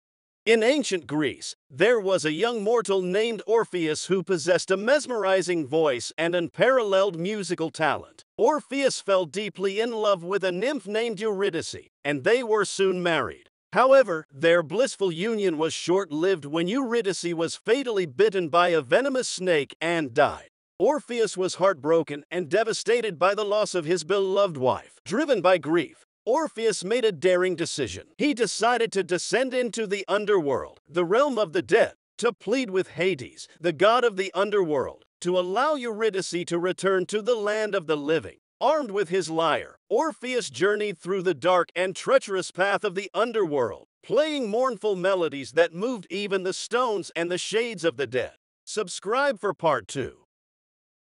Part 1 - The Legend of Orpheus and Eurydice : Lost Love and Melodies of Sorrow
In this captivating retelling of the tragic Greek myth of Orpheus and Eurydice. Follow the journey of Orpheus, a gifted musician who embarks on a perilous quest to bring back his beloved wife, Eurydice, from the Underworld. Will he be able to defy the gods and overcome his own doubts, or will curiosity lead to devastating consequences? This tale of love, loss, and the power of music will tug at your heartstrings and leave you spellbound. greek mythology